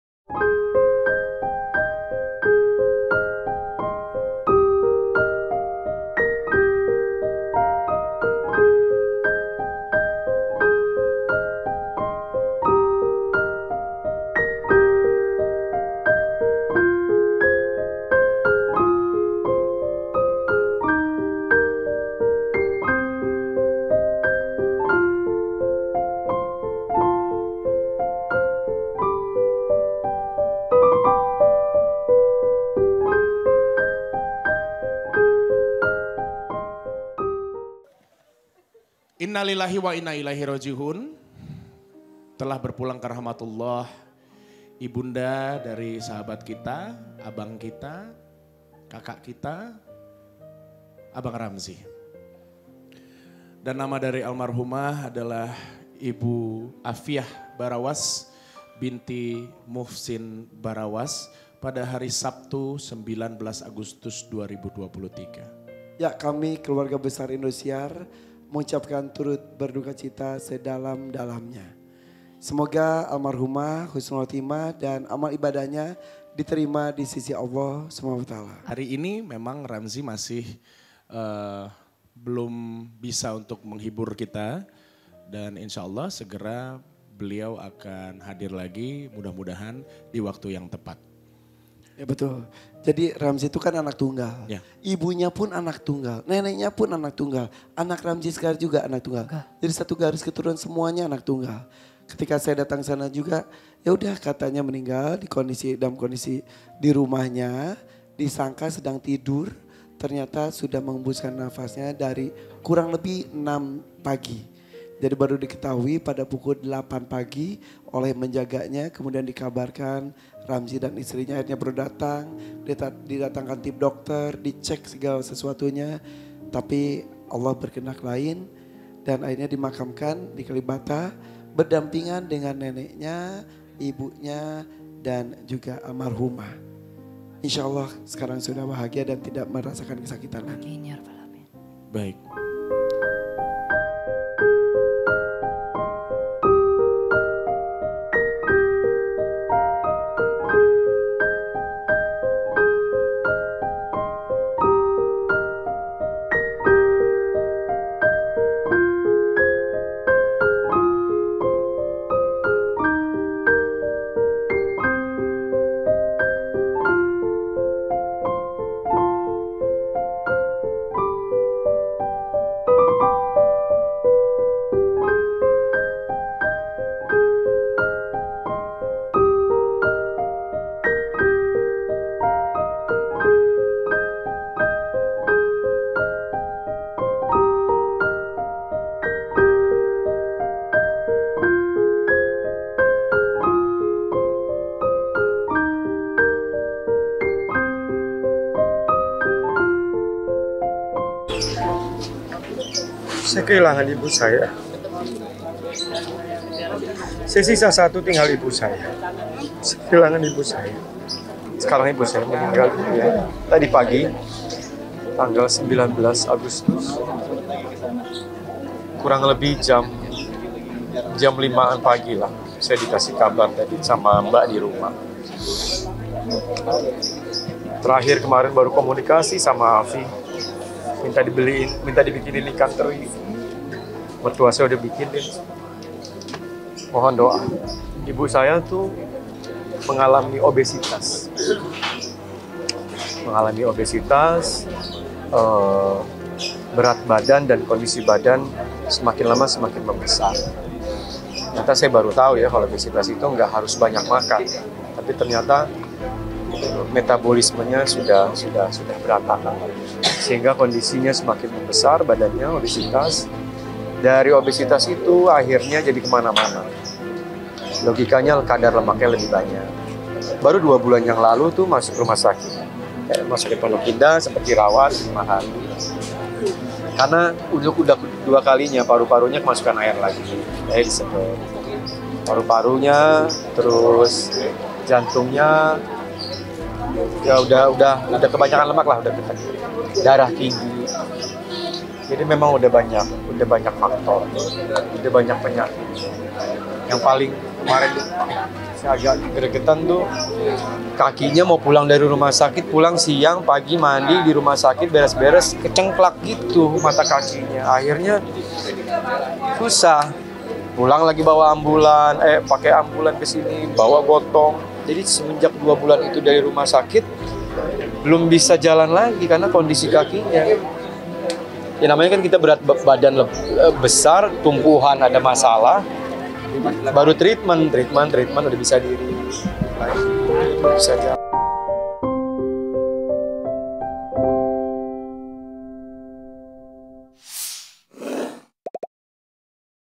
Innalillahi Wainnailaihi Rojiun D'Academy Asia 6 Berduka, Ibunda Ramzi Meninggal Dunia
Merdeka.com - Dangdut Academy Asia 6 tengah berduka. Pasalnya Ibunda Ramzi, Afiah Barawas meninggal dunia pada Sabtu, 19 Agustus 2023, sekitar pukul 06.00 WIB

Ia pun harus absen mengisi sebagai host di Dangdut Academy Asia 6.

Ibunda Ramzi tutup usia 63 tahun dan dimakamkan di pemakaman keluarga di Kalibata, Jakarta Selatan.

Kepergian ibunda Ramzi diduga karena obesitas yang dipicu oleh masalah metabolismen.

Beberapa bulan sebelum wafat, ibundanya sempat dirawat di rumah sakit selama lima hari karena darah tinggi serta paru-paru terendam cairan.

Turut berduka cita, semoga ibunda mendapat tempat terbaik saat ini.

(Ronald)